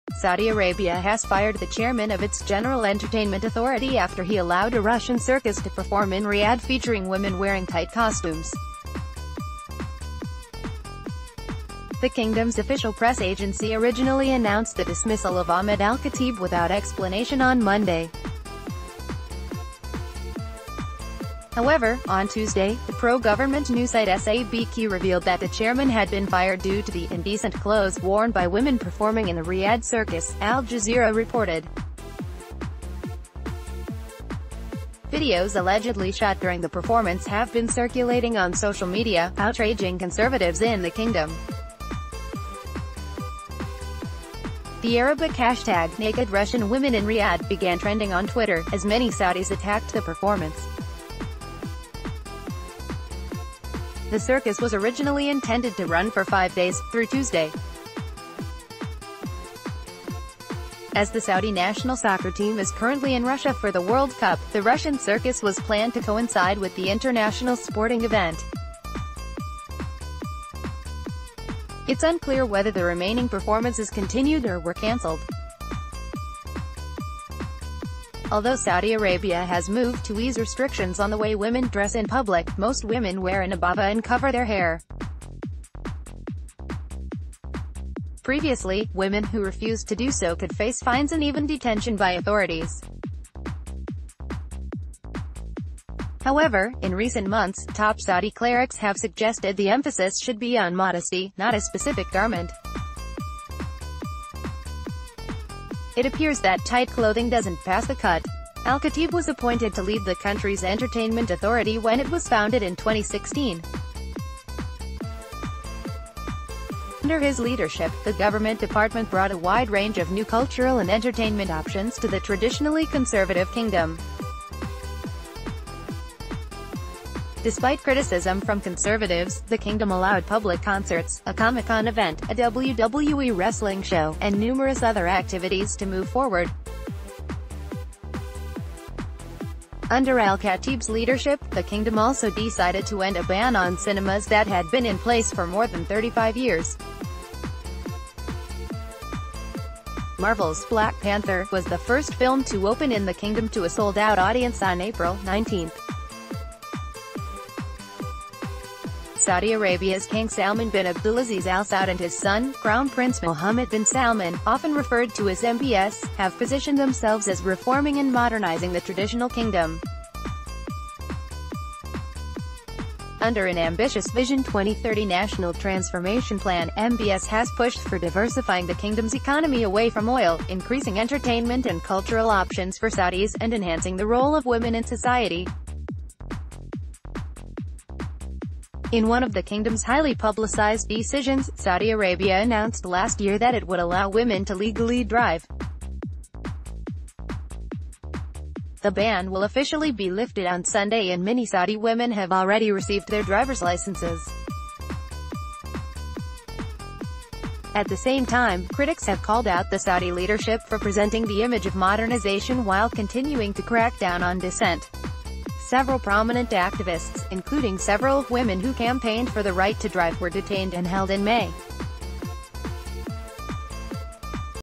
Saudi Arabia Fires Entertainment Chief Because He Allowed Female Performers to Wear Tight Clothes...
Saudi Arabia Fires Entertainment Chief Because He Allowed Female Performers to Wear Tight Clothes Global Nows
Harold Peters